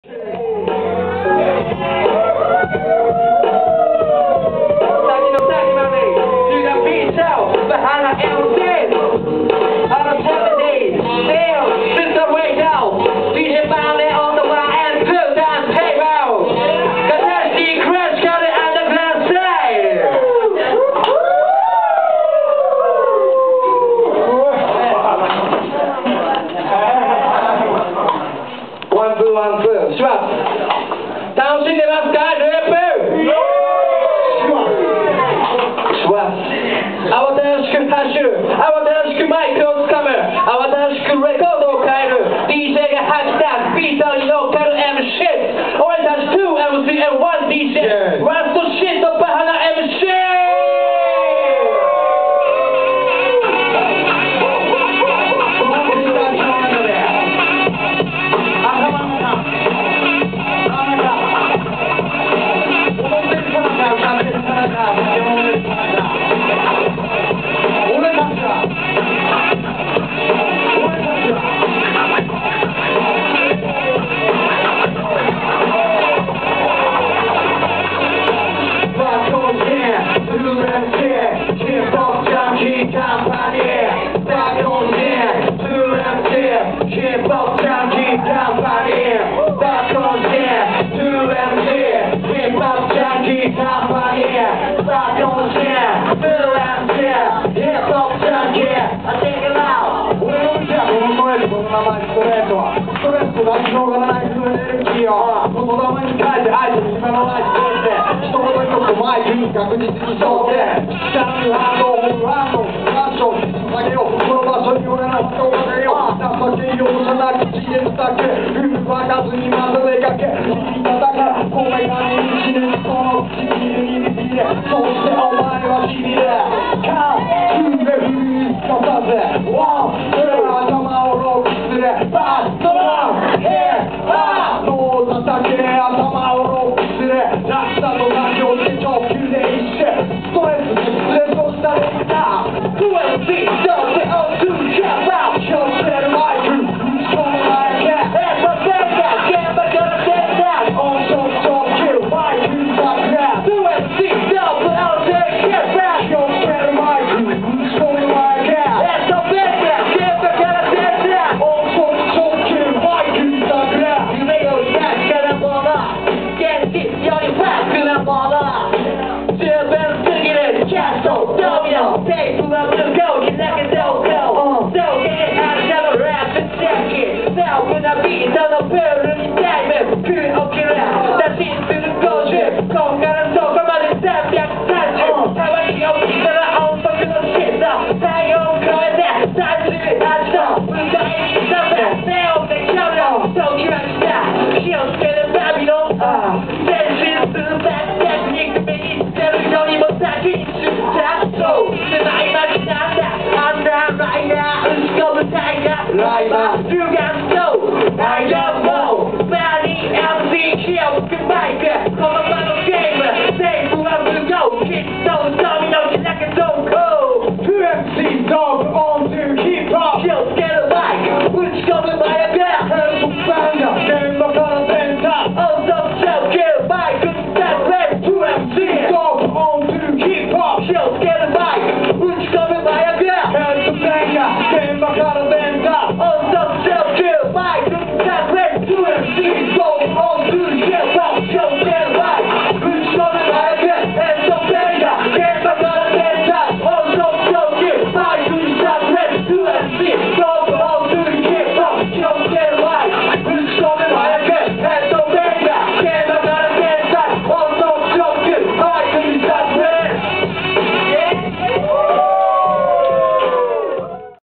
【潜入】BAHANA MC's ② @ LOOP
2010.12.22
『BLACK FILE Vo.2』@ LOOP

《GUEST LIVE》
神門 from 半袖バイブスレコード

《LIVE》
BAHANA MC's
illanika

and more.

《DJ》
And
MANNEN
shige the eye
KAITO

and more.